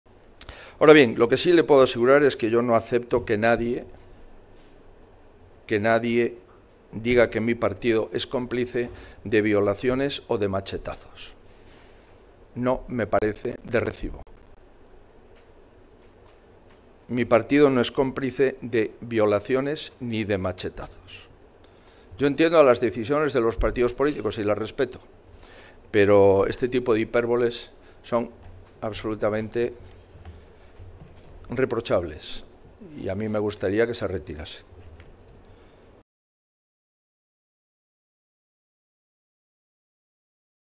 Feijóo se rebela contra Vox: «No acepto que digan que somos cómplices de violaciones y machetazos»
En referencia a algunas de las aseveraciones de dirigentes de Vox, ha afirmado que «no acepto que nadie» diga que su partido es «cómplice de violaciones y de machetazos», ya que «no me parece de recibo».

Video: PP